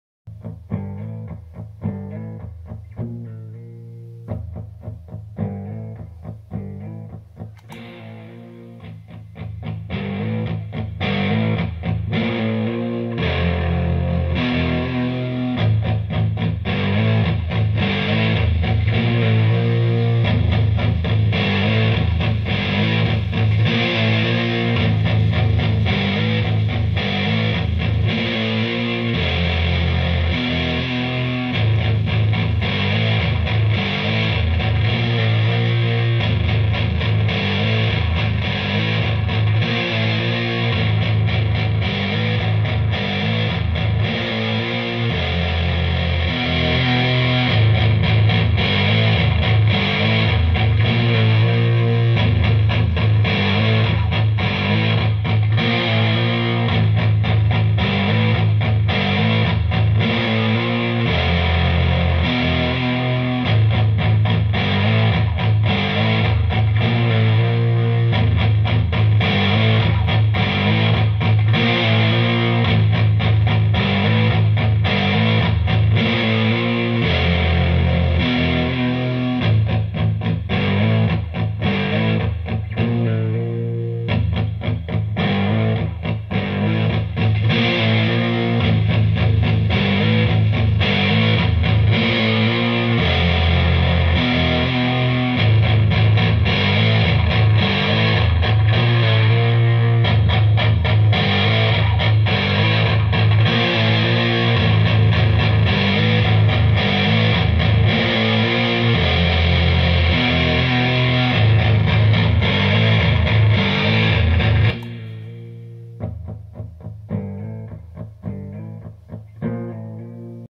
Witchfinder Effects Black Lodge Distortion Demo
This is a quick demo clip of the Black Lodge distortion by Witchfinder Effects. It is a Vaccaro Generator X tuned to Drop A into the Black Lodge into a squeaky clean fender amp. (Pardon the shitty cell phone mic)